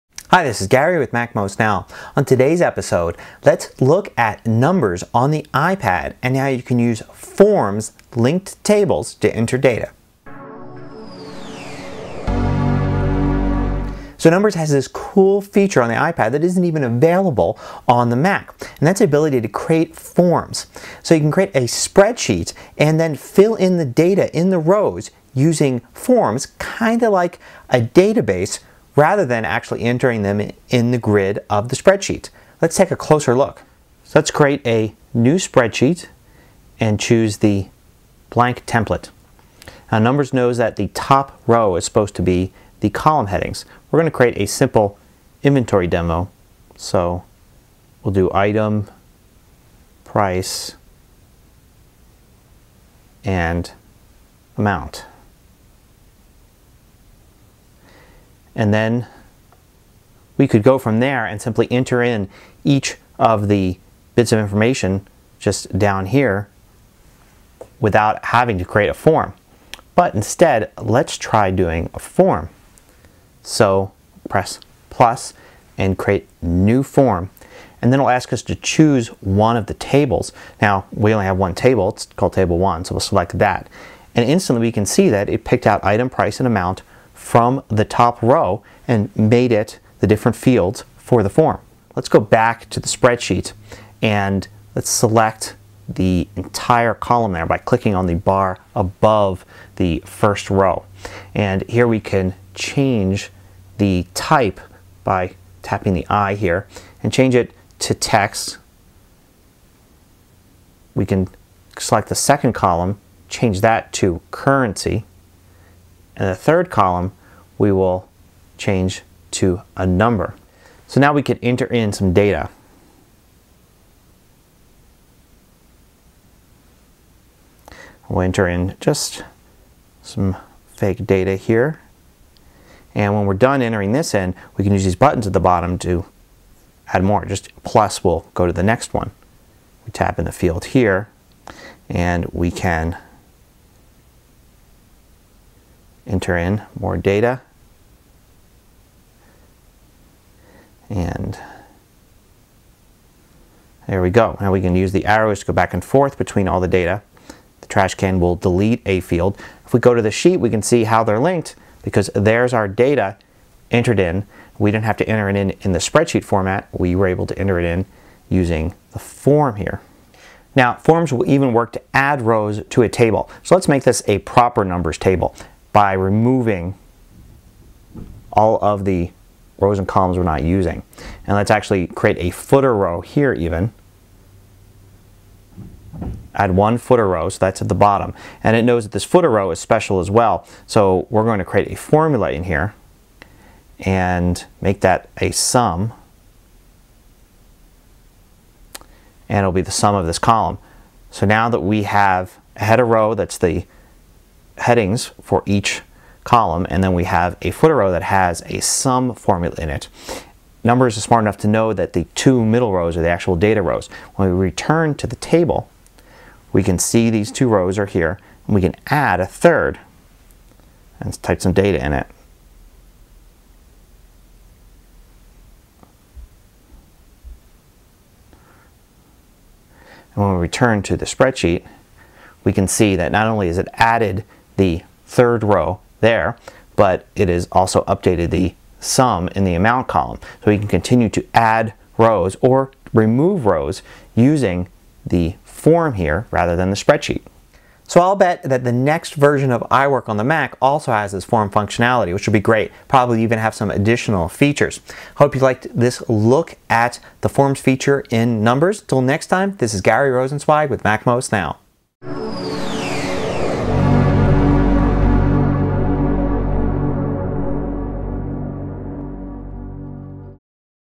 Creating Forms In iPad Numbers (MacMost Now 398)
One interesting feature of Numbers on the iPad is the ability to create forms. You can use forms to fill in the rows of a spreadsheet instead of entering data into the spreadsheet grid.